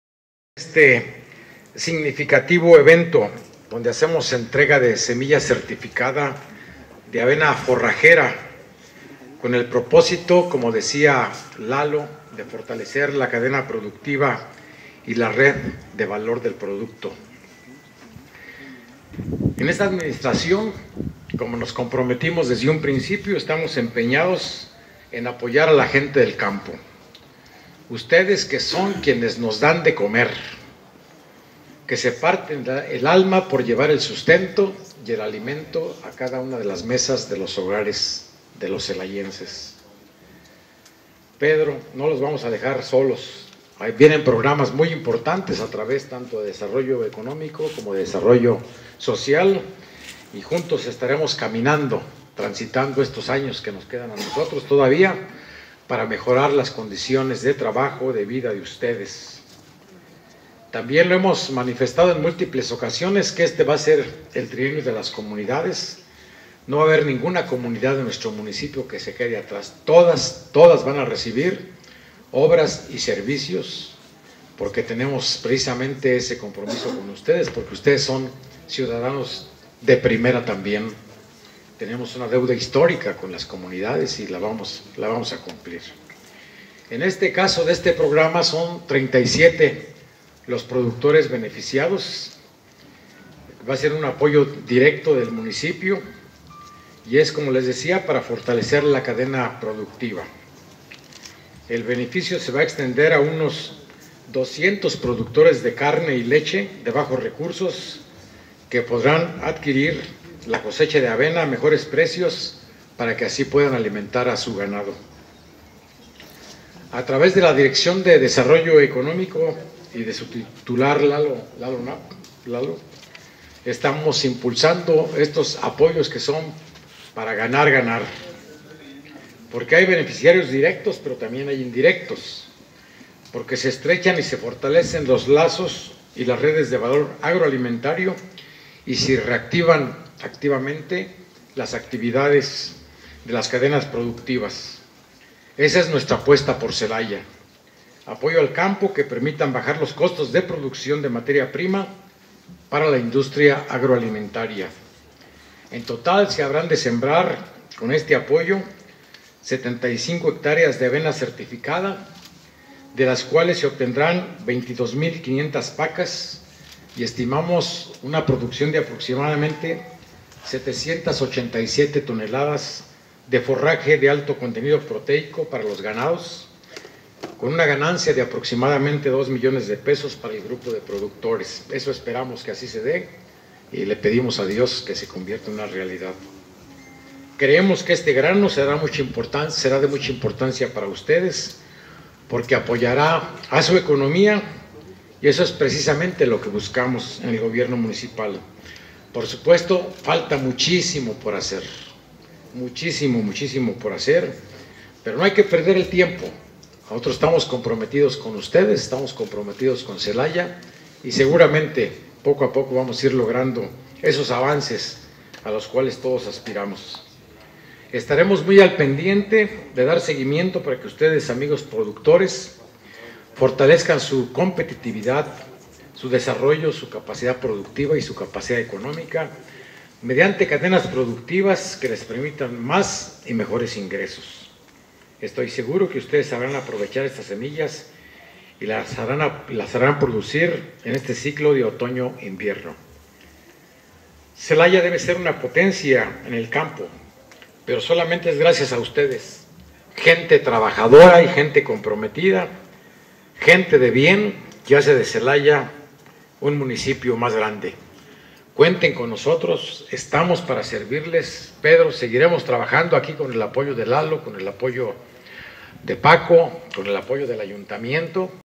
ENTREGAN AUTORIDADES MUNICIPALES APOYOS PARA GANADEROS
Recibieron semilla certificada de avena forrajera para el cultivo de alimento para el ganado.

Para continuar con la reactivación económica de las cadenas productivas, el presidente municipal, Javier Mendoza Márquez, entregó costales de semilla certificada de avena forrajera, a integrantes de la Asociación Ganadera Local.